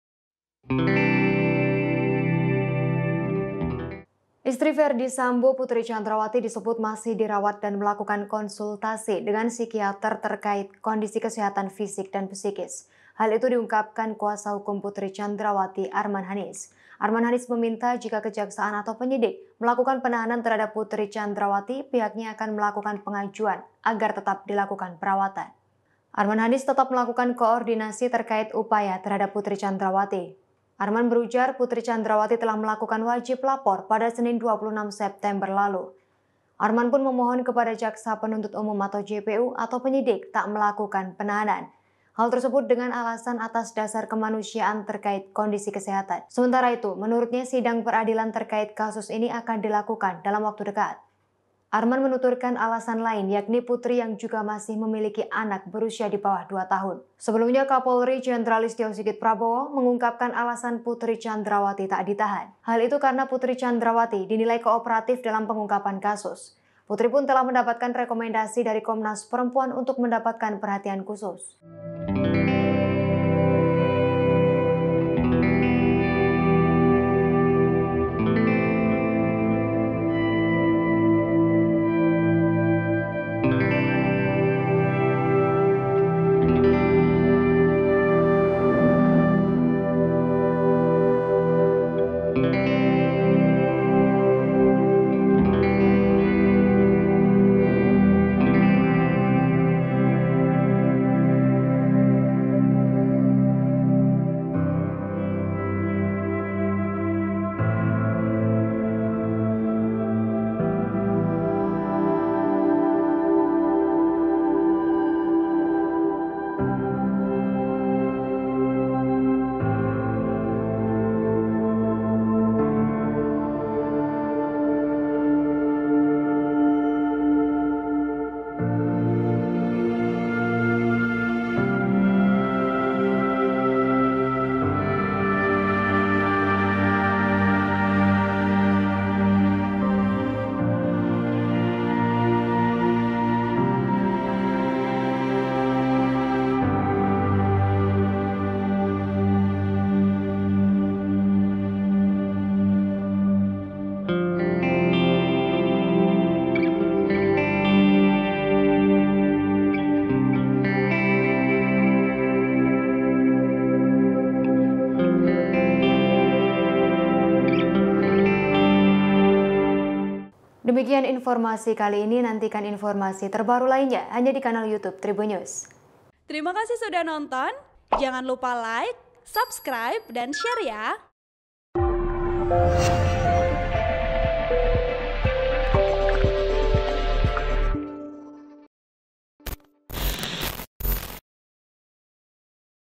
Ungkap Kondisi Kesehatan Putri Candrawathi, Pengacara: Masih Dirawat dan Konsultasi dengan Psikiater
TRIBUN-VIDEO.COM - Istri Ferdy Sambo, Putri Candrawathi disebut masih dirawat dan melakukan konsultasi dengan psikiater terkait kondisi kesehatan fisik dan psikis.

Hal itu diungkapkan Kuasa hukum Putri Candrawathi, Arman Hanis.

Arman Hanis meminta, jika Kejaksaan atau penyidik melakukan penahanan terhadap Putri Candrawathi, pihaknya akan melakukan pengajuan agar tetap dilakukan perawatan.

Arman Hanis tetap melakukan koordinasi terkait upaya terhadap Putri Candrawathi.

"Apabila pihak Kejaksaan atau penyidik melakukan penahan, kami akan berkoordinasi untuk tetap dapat dilakukan perawatan," ujar Arman.

Arman berujar, Putri Candrawathi telah melakukan wajib lapor pada Senin (26/9/2022) lalu.

Arman pun memohon kepada Jaksa Penuntut Umum (JPU) atau penyidik tak melakukan penahanan.

Hal tersebut dengan alasan atas dasar kemanusiaan terkait kondisi kesehatan.

Sementara itu, menurutnya sidang peradilan terkait kasus ini akan dilakukan dalam waktu dekat.

"Selaku tim kuasa hukum, tentu memohon kepada penyidik atau JPU agar dapat mempertimbangkan alasan-alasan kemanusiaan yaitu kondisi kesehatan klien kami, khususnya menjelang proses peradilan."

Arman menuturkan alasan lain yakni Putri yang juga masih memiliki anak berusia di bawah 2 tahun.

Sebelumnya, Kapolri Jenderal Listyo Sigit Prabowo mengungkapkan alasan Putri Candrawathi tak ditahan.

Hal itu karena Putri Candrawathi dinilai kooperatif dalam pengungkapan kasus.

Putri pun telah mendapatkan rekomendasi dari Komnas Perempuan untuk mendapatkan perhatian khusus.

(Tribun-Video.com/ Tribunnews.com)

Artikel ini telah tayang di Tribunnews.com dengan judul Pengacara: Putri Candrawathi Masih Dirawat dan Konsultasi dengan Psikiater,